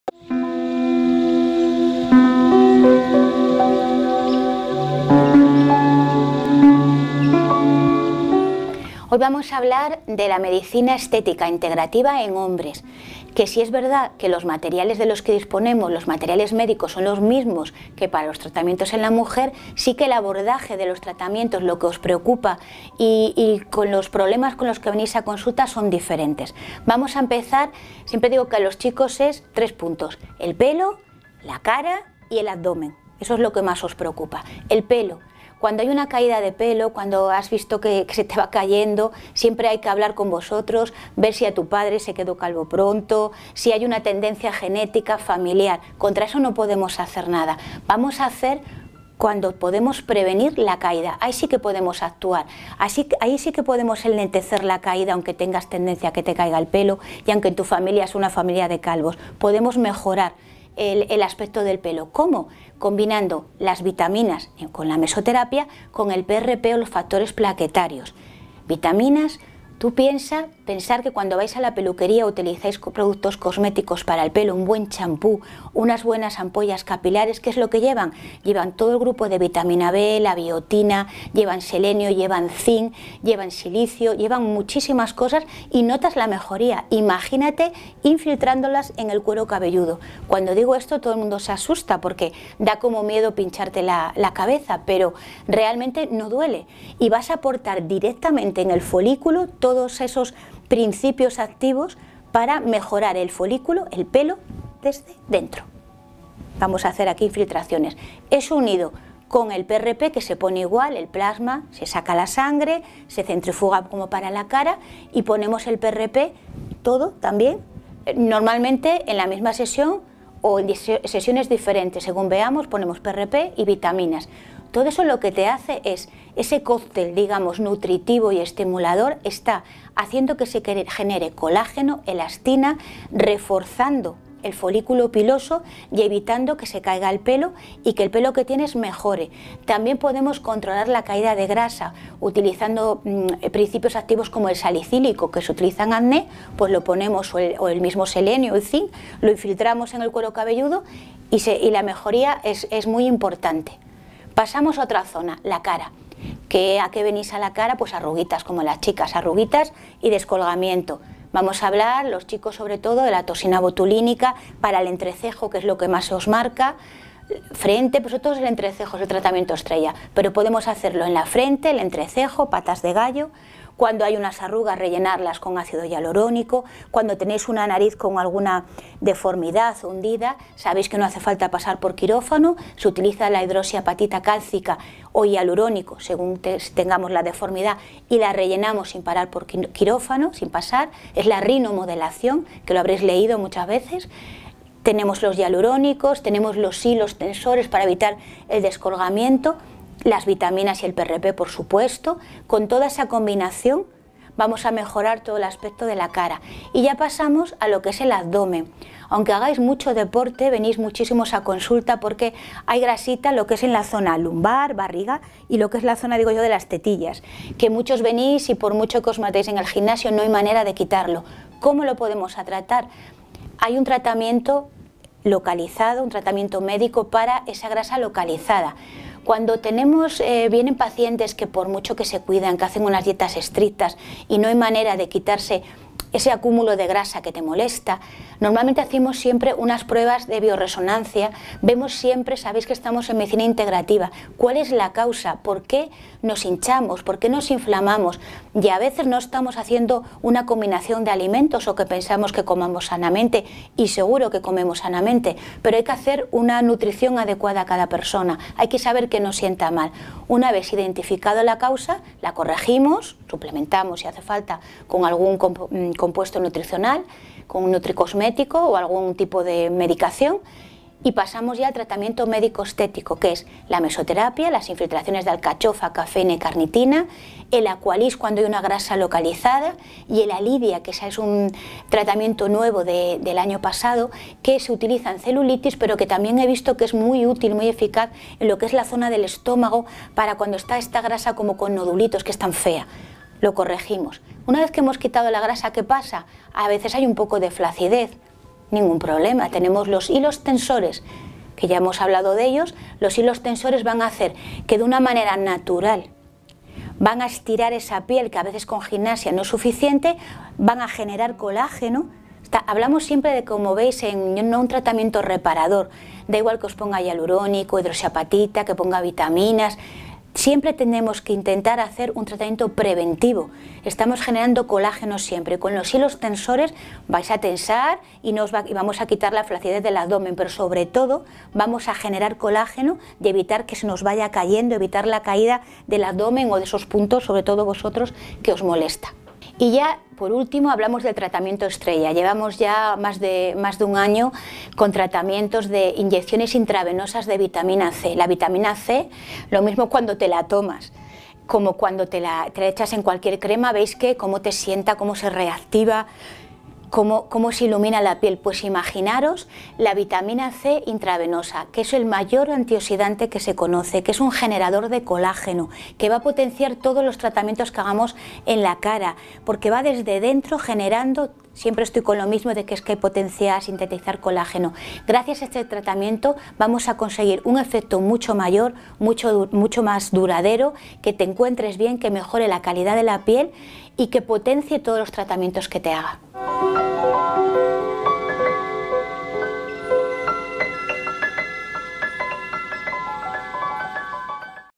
MEDICINA ESTÉTICA INTEGRATIVA PARA HOMBRES
La Dra. Maroto nos habla sobre la medicina estética integrativa para hombres. Una serie de tratamientos preparados especialmente para hombres.